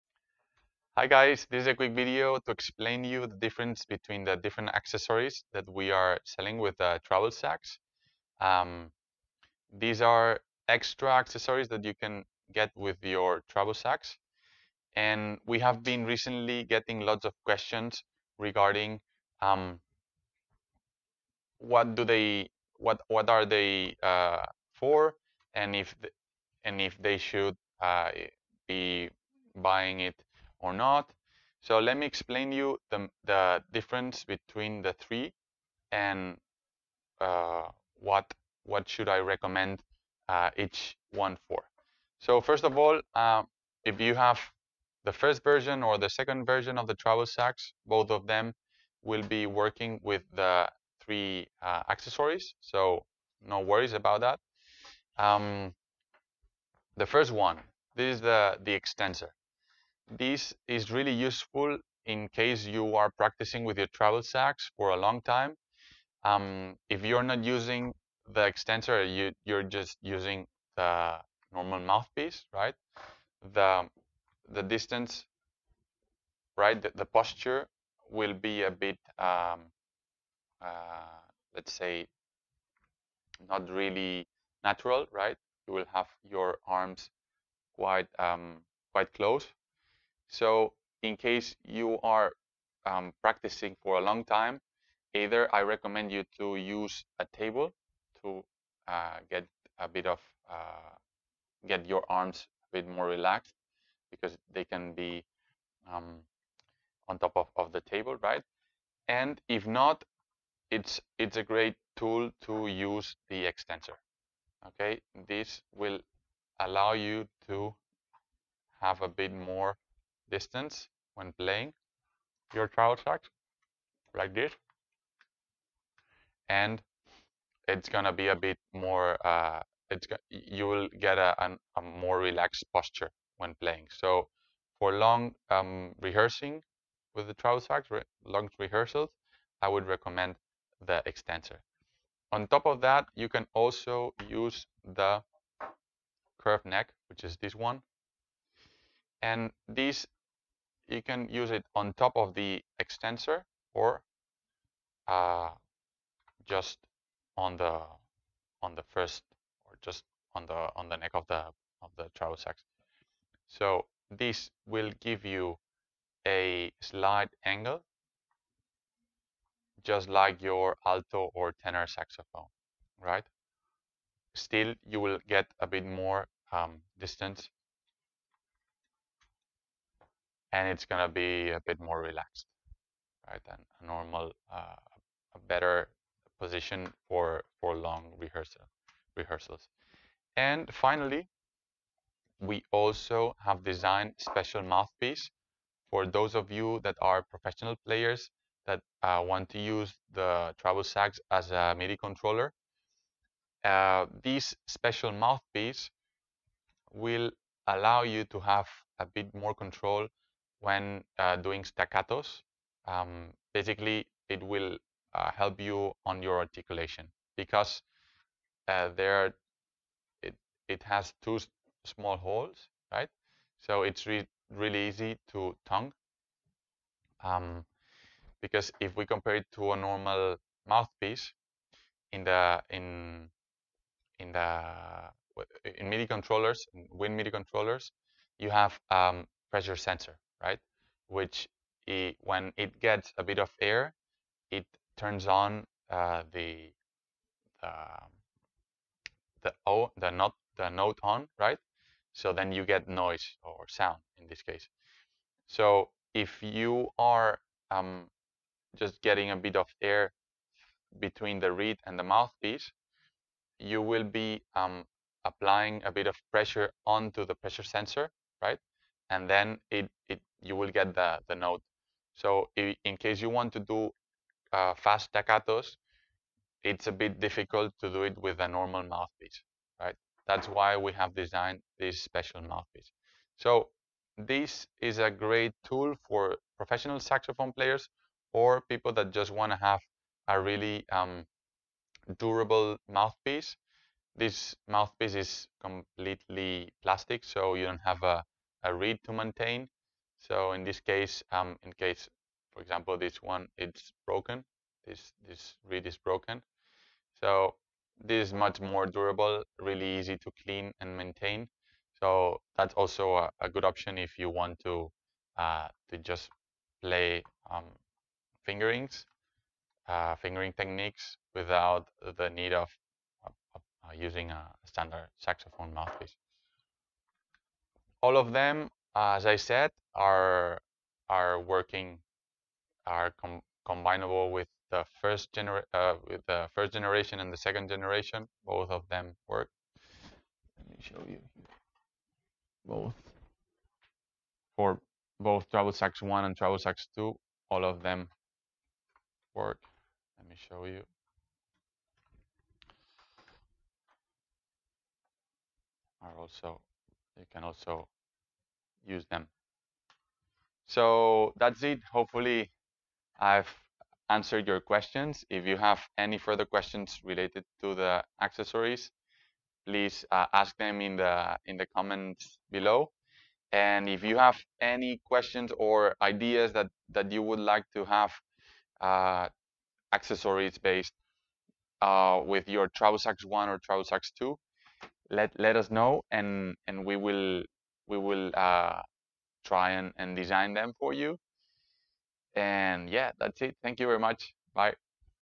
Travel Sax accesories - Which should I use?
In this video we will show you the difference between the 3 Travel Sax accesories and what are they used for.

Please do not hesitate to write any question you might have in the comments section bellow.

❔Schedule a Quick Meeting with the creator of Travel Sax!!

📦 Now shipping worldwide!!